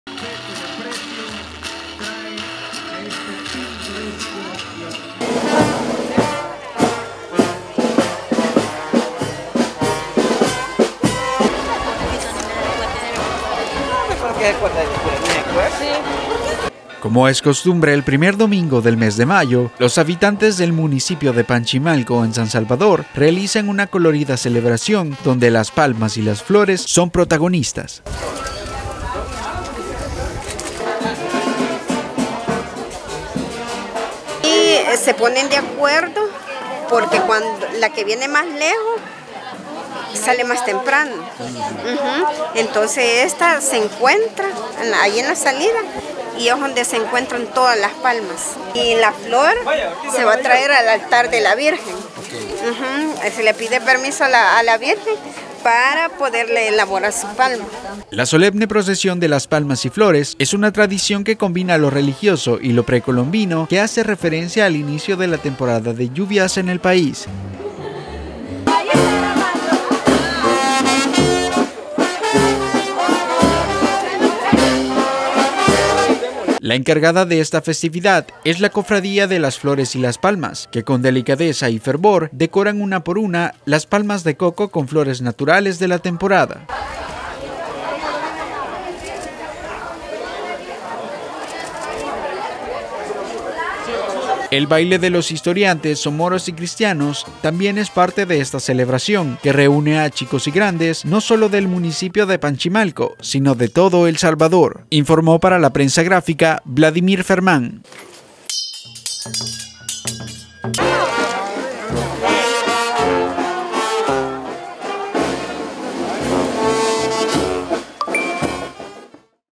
Una colorida tradición vistió de palmas y flores las calles de Panchimalco
Como es costumbre el primer domingo del mes de mayo los habitantes del municipio de Panchimalco en  San Salvador, realizan una colorida celebración donde las palmas y las flores son protagonistas.